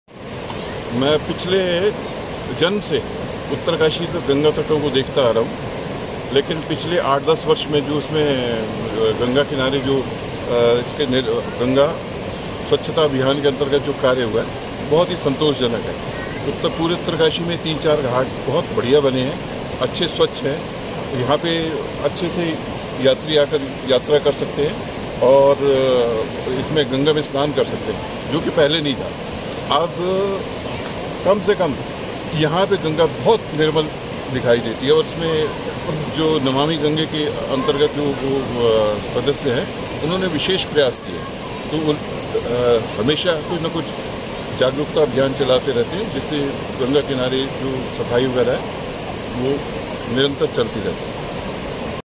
विगत वर्षों में नमामिगंगे अभियान के अंतर्गत जो कार्य हुए वो बेहद संतोषजनक हैं - निवासी, उत्तरकाशी
विगत वर्षों में नमामि गंगे अभियान के अंतर्गत जो कार्य हुए हैं वो बेहद संतोषजनक हैं   – स्थानीय निवासी, उत्तरकाशी